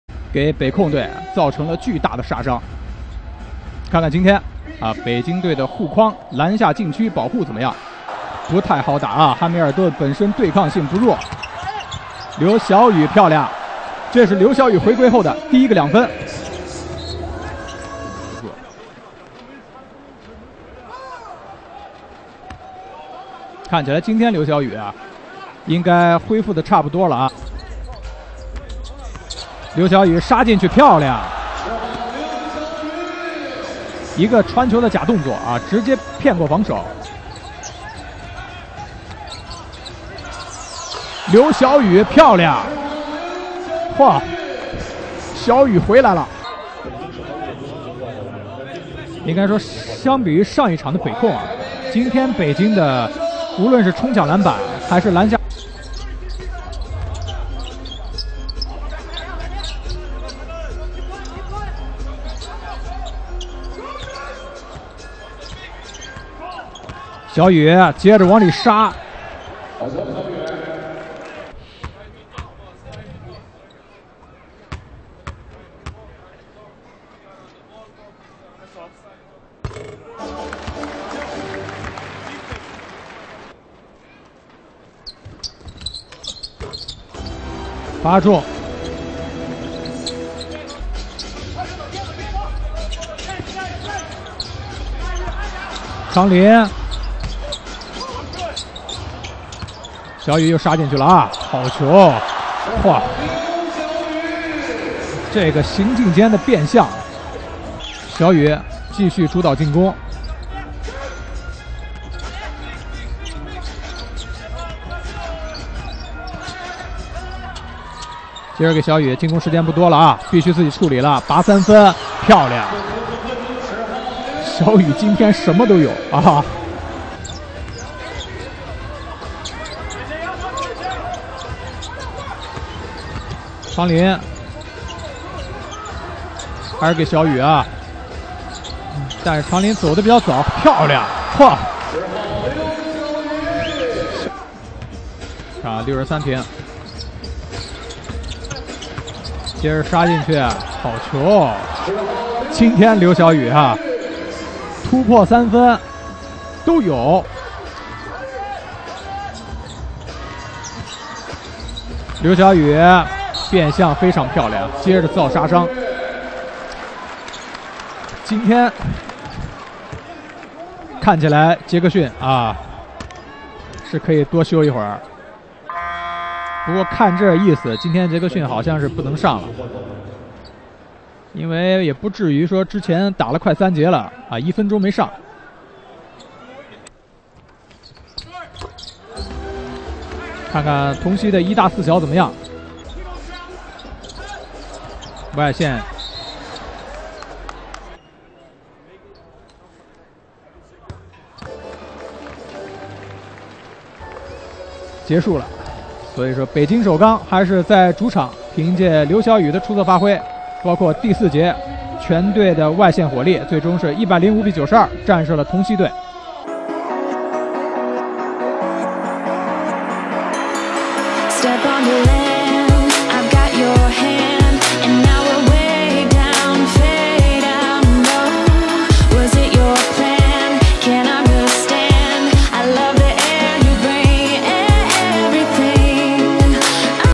[中国篮球]状态回勇！CBA北京刘晓宇21分vs南京同曦集锦|17.12.26
方硕和刘晓宇，北京队幸福的烦恼！